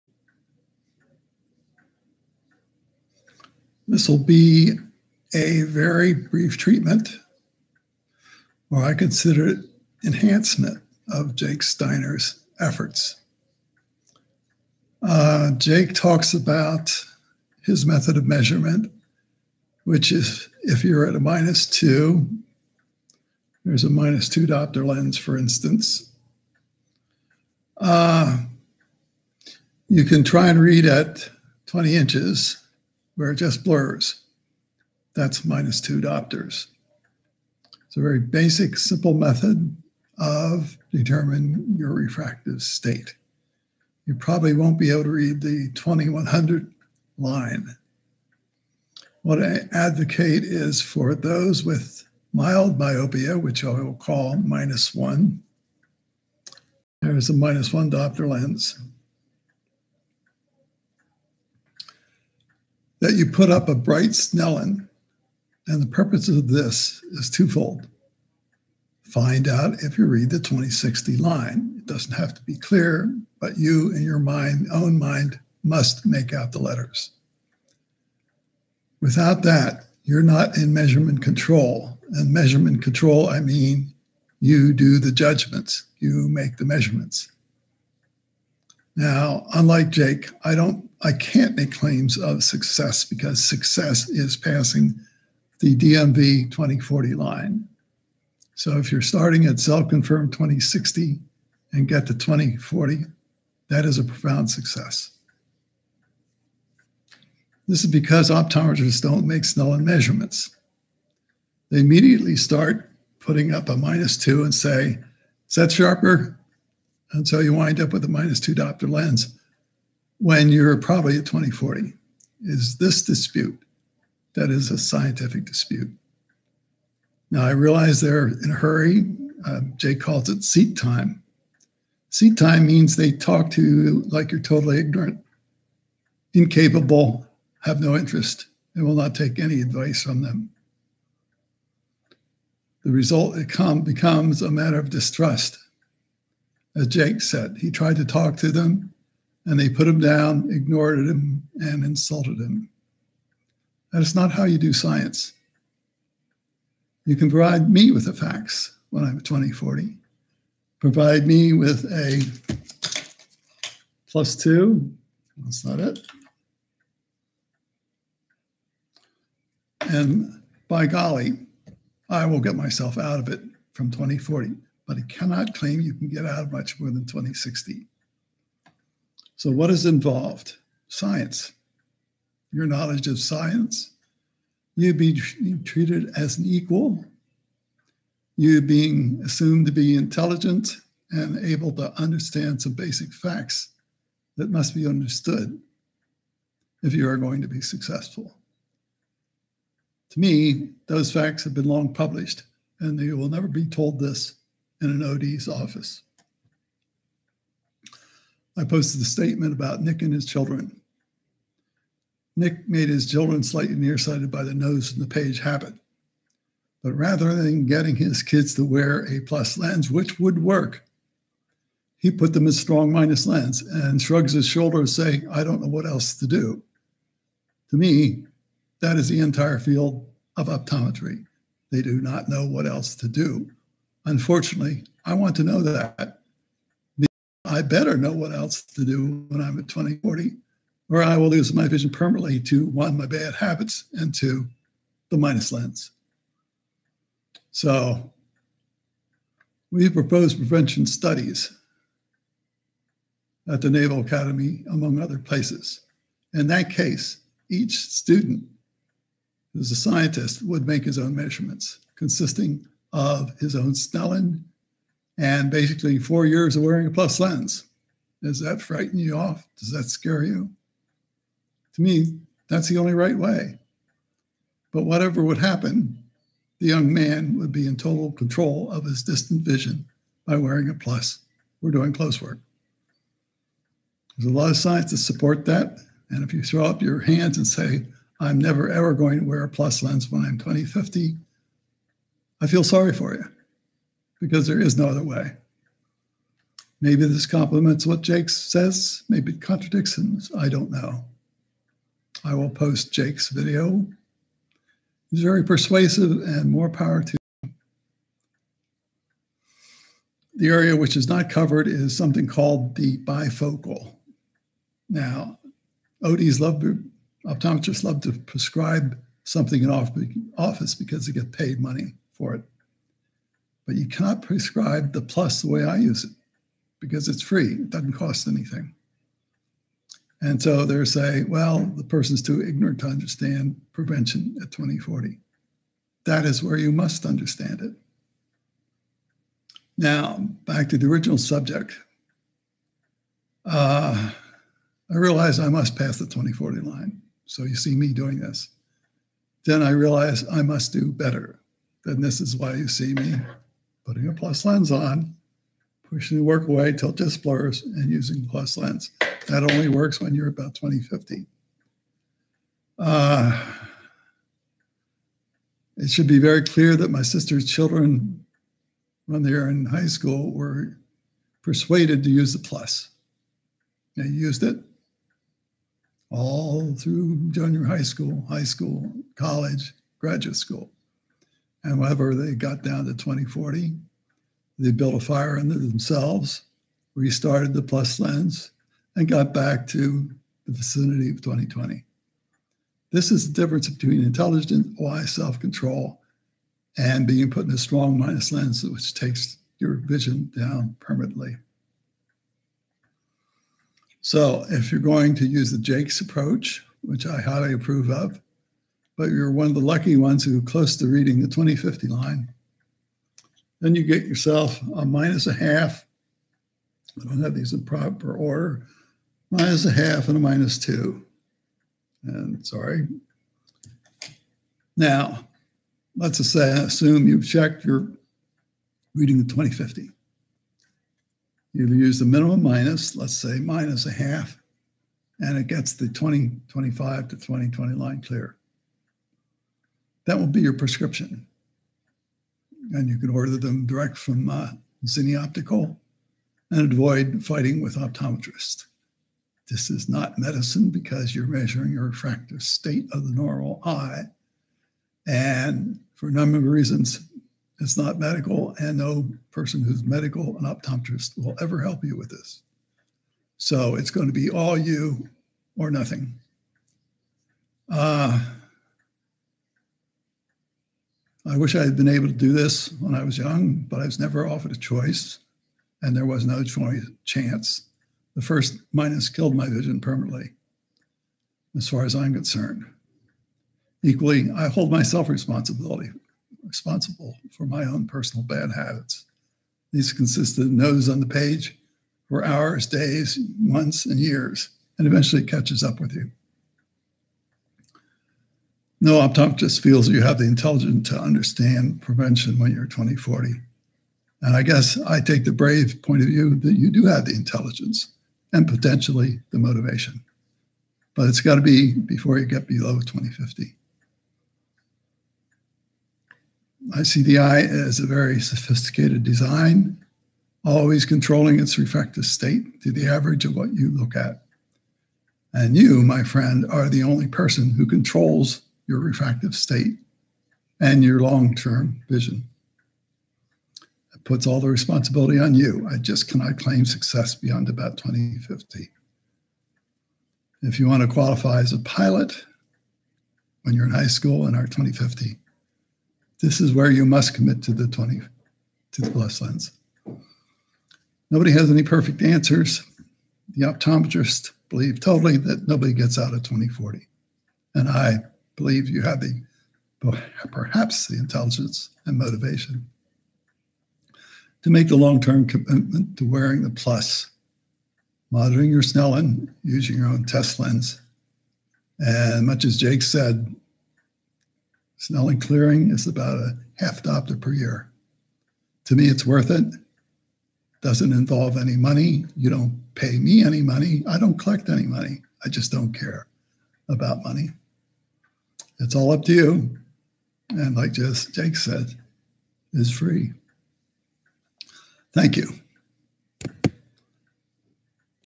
Self Measure of Your Refractive State.
The second-opinion is that the eye is a dynamic system, and that in the early stage, prevention is possible.  These videos are designed to help you understand the type of choice you have, if you still read the 20/50 line (about -1.0 diopters), and wish to change your refractive state by +1.0 diopters and return to 20/20. This is to support a proposed scientific study - under your control.